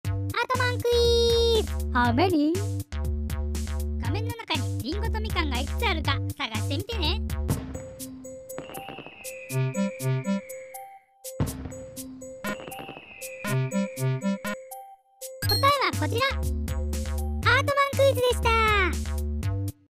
何かがいくつあるかの映像クイズ Artman VisualQuiz#781
This program is picture quiz. Look at monitor, How many "Apples" can you find?

画面に出ている【特定の物体】を探して下さい。。

YamazakiMediaMix Co.,Ltd.